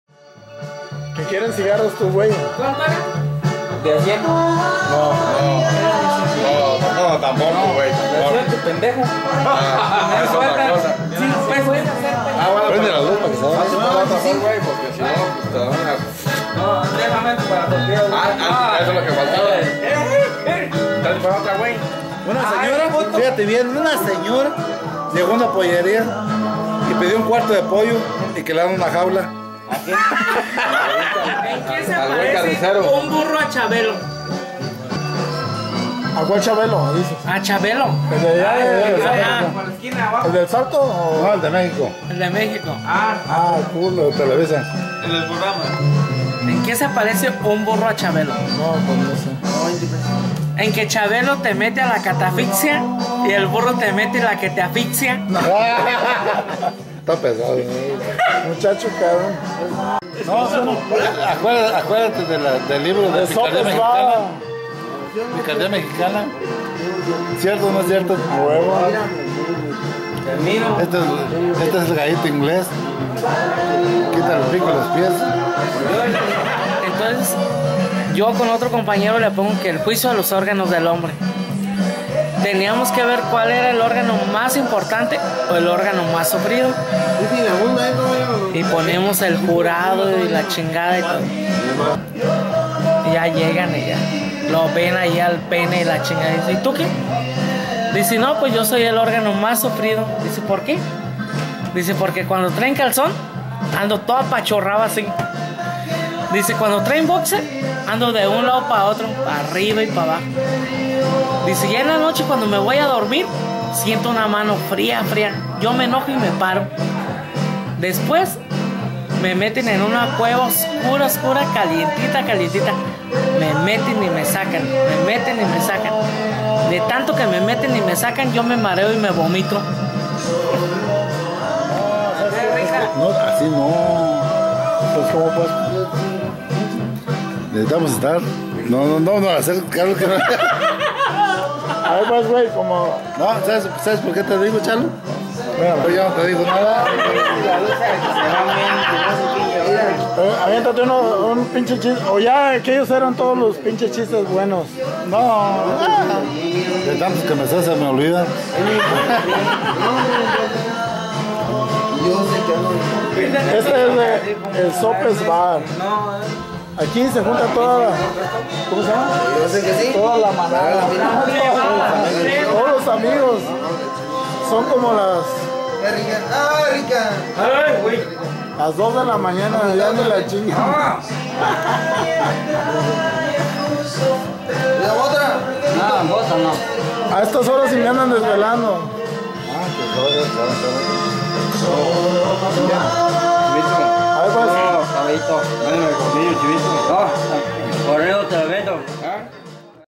The Sopes's Bar en Parácuaro, Mich.
Este es el ambiente nocturno de Parácuaro, Michoacán. En uno de los lugares más concurridos a altas horas de la noche. The Sopes´s Bar, donde disfrutas tus bebidas, acompañado de los amenos chistes de Lalo, y de los visitantes.